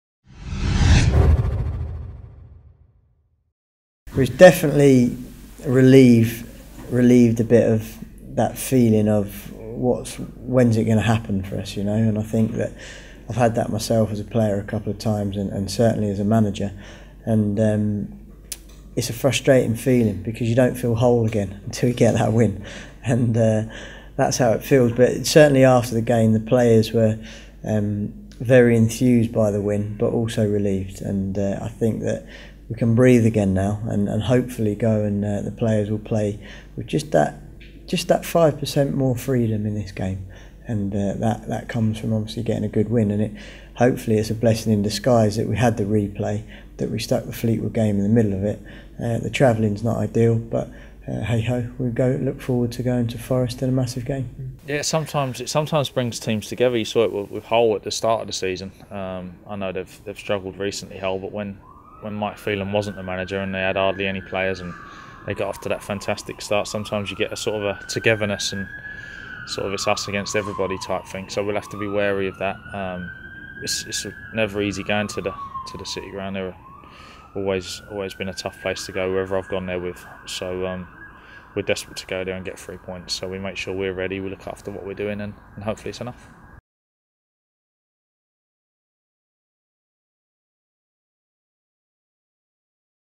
Nottingham Forest v Bristol City Pre-Match Interviews
Lee Johnson and Gary O'Neil discuss City's trip to the City Ground.